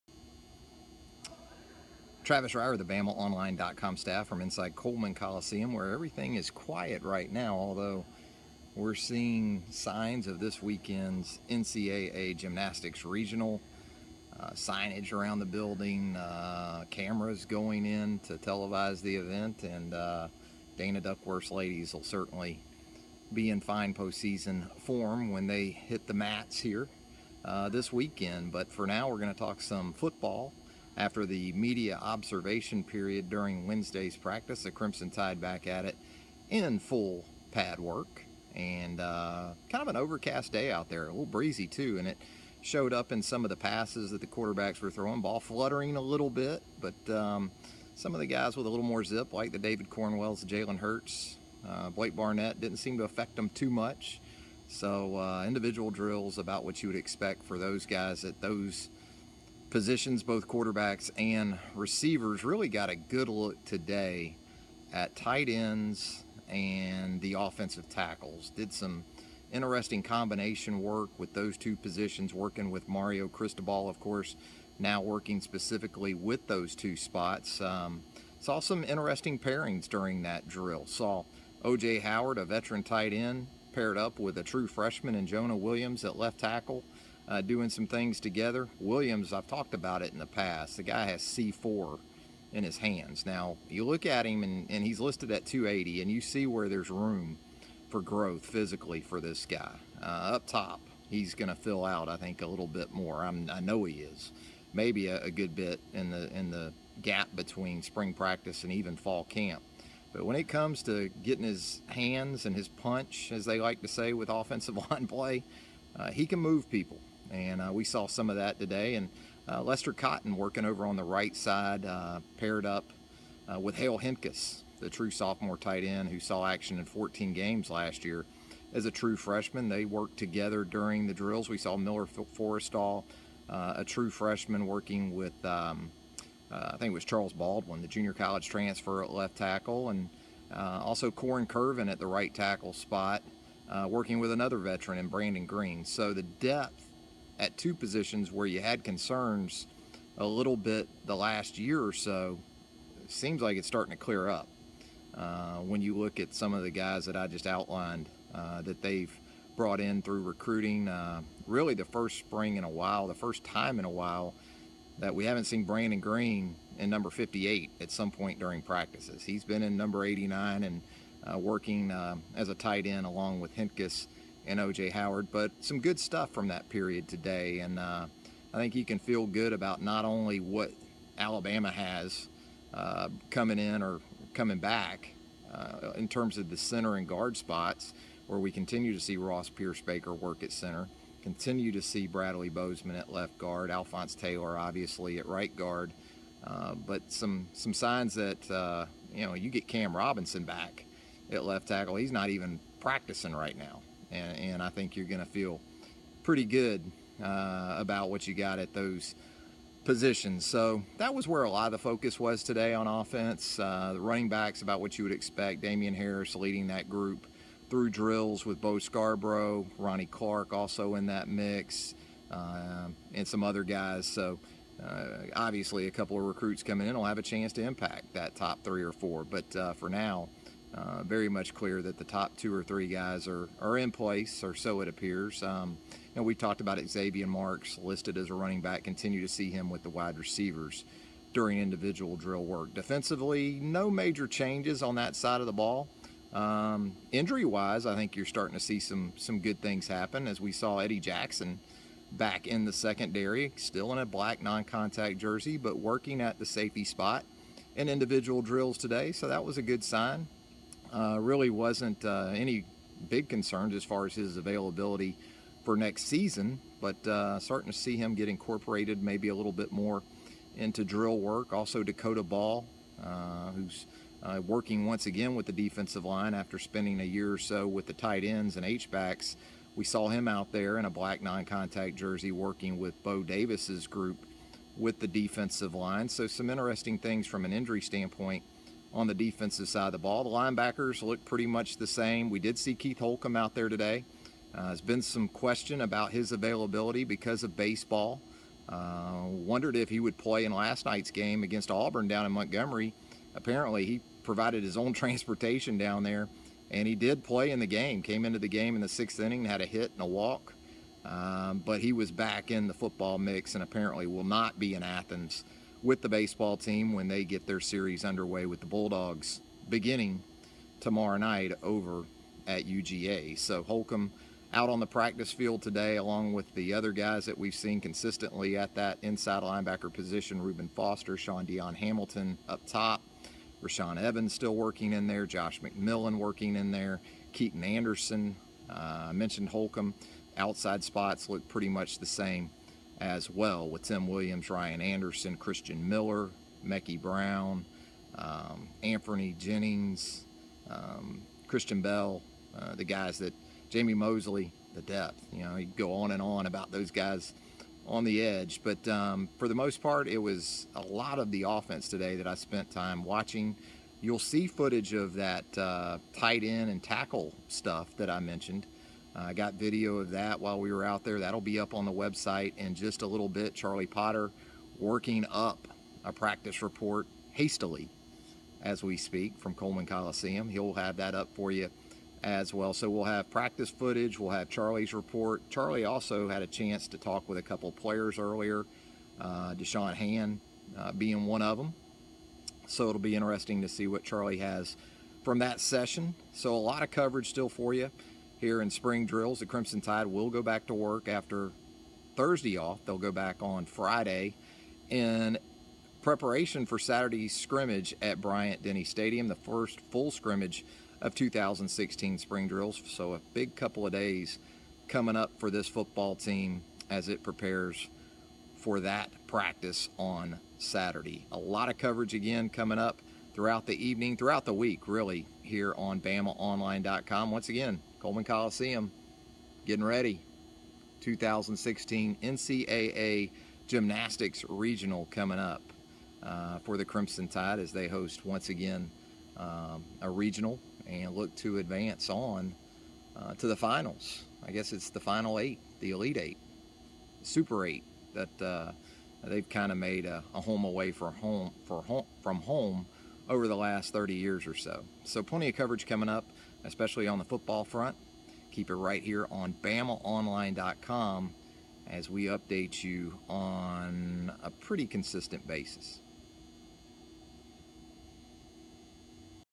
Instant Analysis: Wednesday, March 30, 2016
BOL senior analyst Travis Reier talks Alabama football.